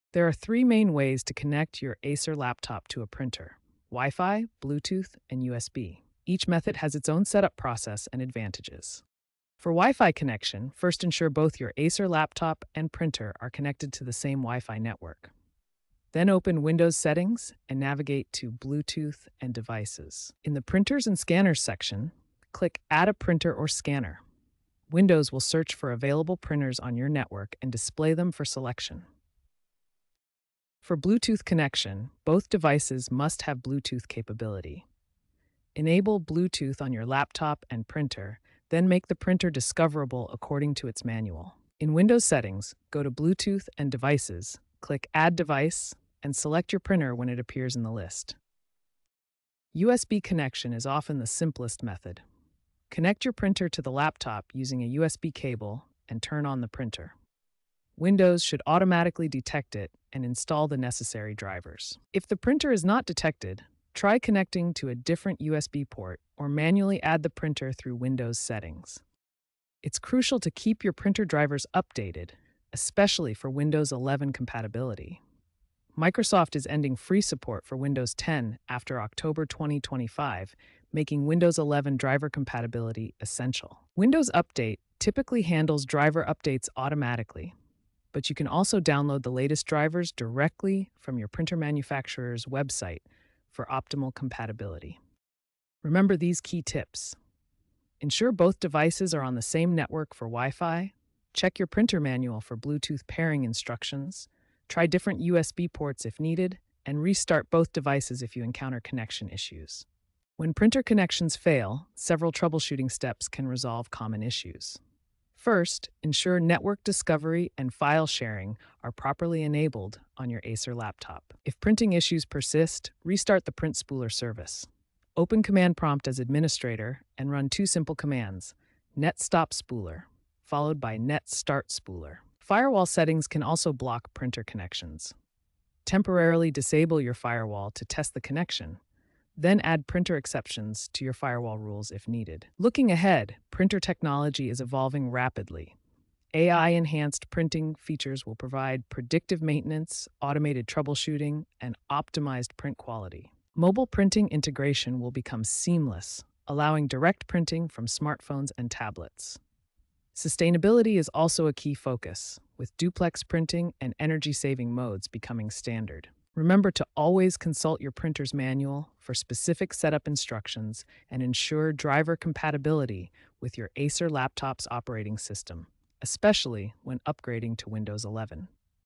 How To Connect Acer Laptop To Printer – WiFi, Bluetooth, And USB Setup
Are you struggling to connect your Acer laptop to a printer? Look no further! In this detailed video, we will walk you through the various methods of connecting your device, including WiFi, Bluetooth, and USB options. Our step-by-step instructions are designed to make the setup process easy and efficient, regardless of your technical expertise.